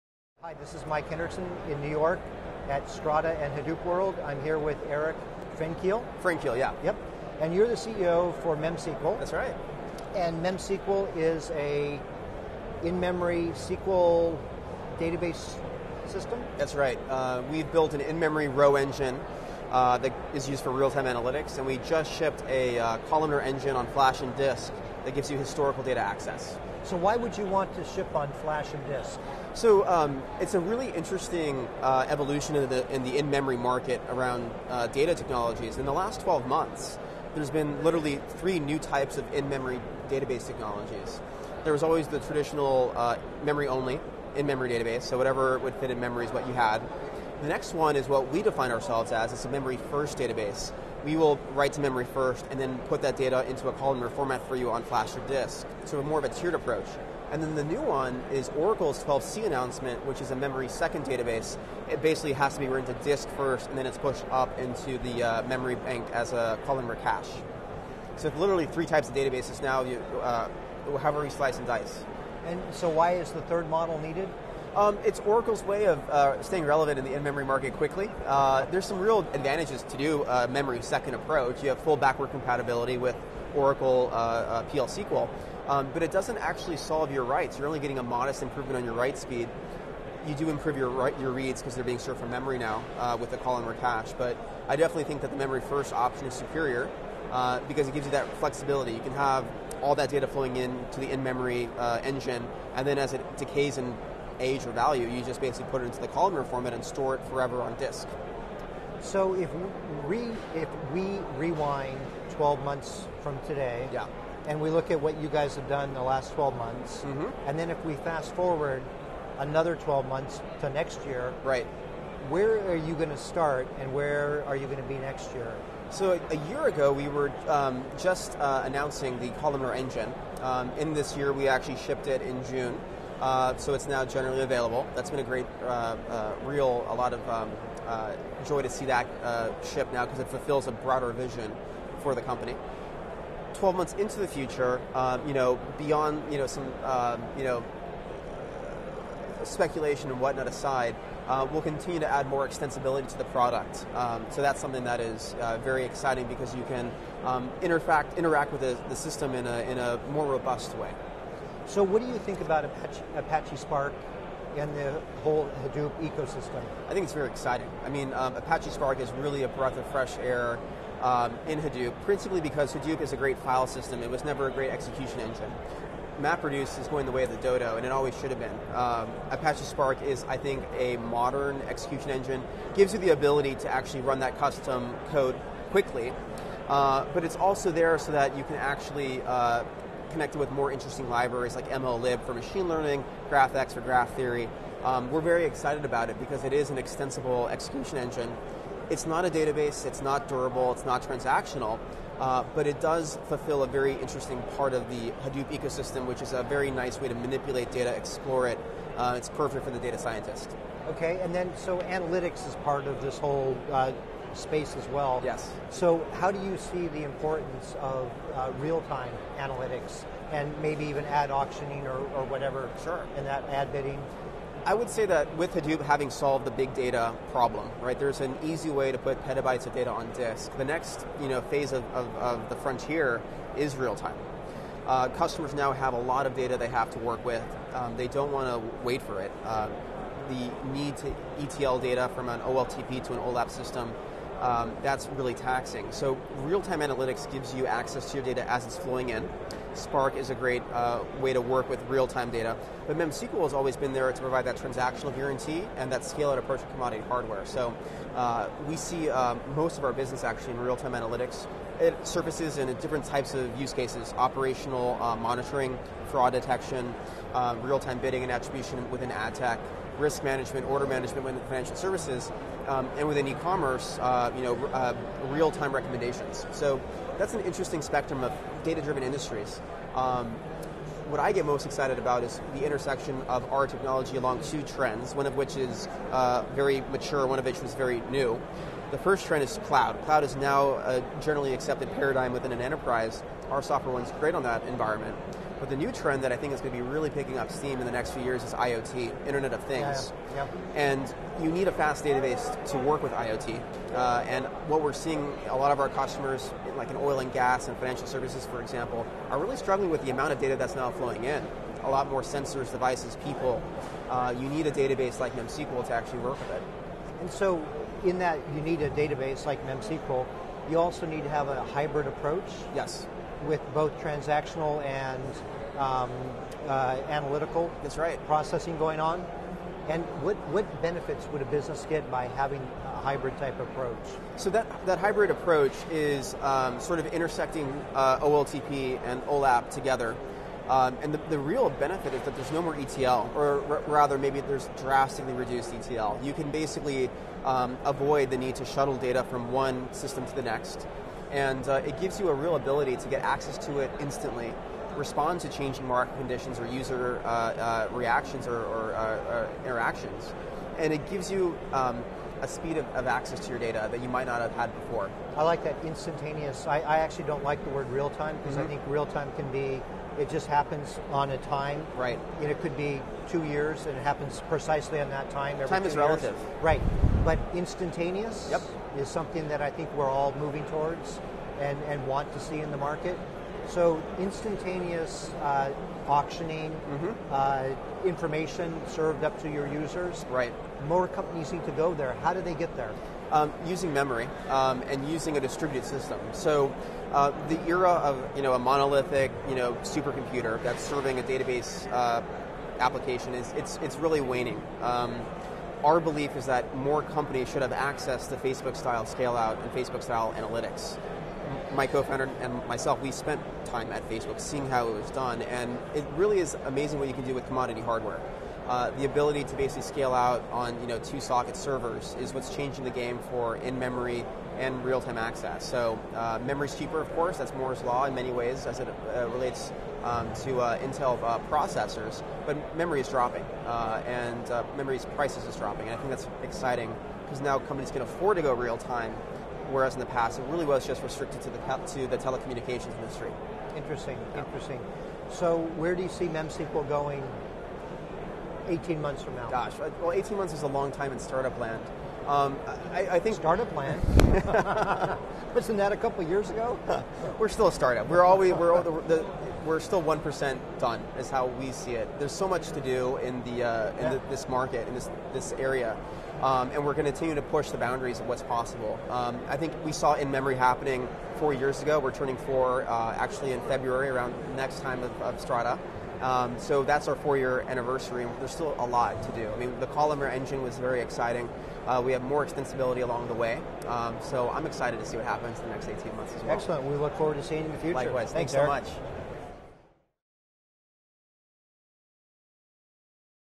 Eric Frenkiel (MemSQL) Interview -- Strata + Hadoop 2014
From the 2014 Strata Conference + Hadoop World in New York City, an interview with the CEO of MemSQL about their in-memory SQL database system and the importance of real-life or even instantaneous analytics.

About Eric Frienkiel:
Eric is the cofounder and CEO of MemSQL, the leader in real-time and historical Big Data analytics based on a distributed in-memory database. Before MemSQL, Eric worked at Facebook on partnership development. He has worked in various engineering and sales engineering capacities at both consumer and enterprise startups. Eric is a graduate of Stanford University’s School of Engineering. In 2011 and 2012, Eric was named to Forbes’ 30 under 30 list of technology innovators.